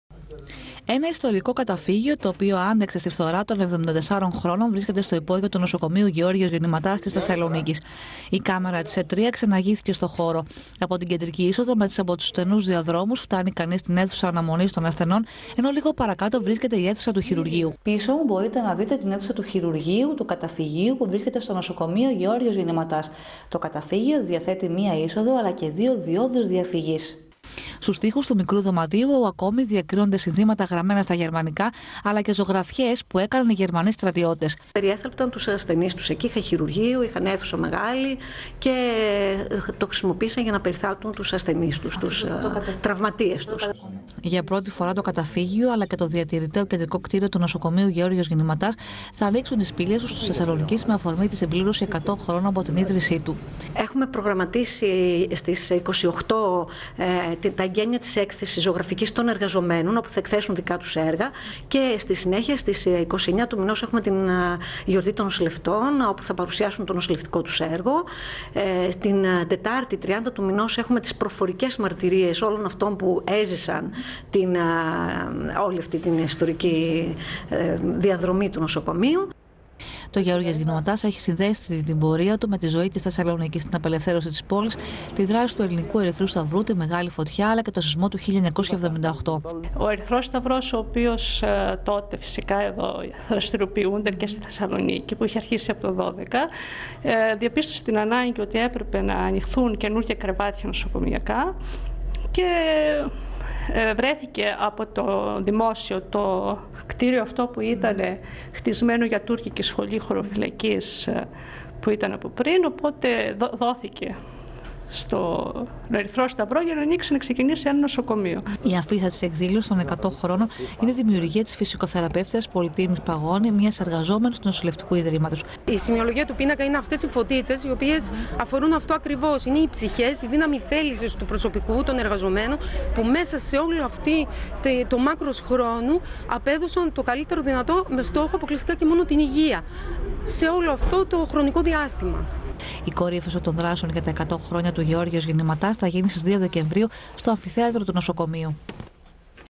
100 χρόνια λειτουργίας κλείνει το νοσοκομείο Γεννηματάς
Για πρώτη φορά ,στην ιστορία του ,το καταφύγιο, αλλά και το διατηρητέο κεντρικό κτίριο του νοσηλευτικού ιδρύματος «Γ. Γεννηματάς», θα ανοίξουν τις πύλες τους στους Θεσσαλονικείς, με αφορμή τη συμπλήρωση 100 χρόνων από την ίδρυση του νοσοκομείου. Πρόκειται για ένα καταφύγιο το οποίο έχτισαν το 1942 οι γερμανοί κατακτητές και το χρησιμοποιούσαν ως νοσοκομείο για να περιθάλπουν τους τραυματίες τους.Για τη συμπλήρωση των 100 χρόνων από την ίδρυση του νοσοκομείου έχουν προγραμματιστεί και εκδηλώσεις

ΡΕΠΟΡΤΑΖ ΦΡΟΣΩ ΒΑΡΒΑΡΑ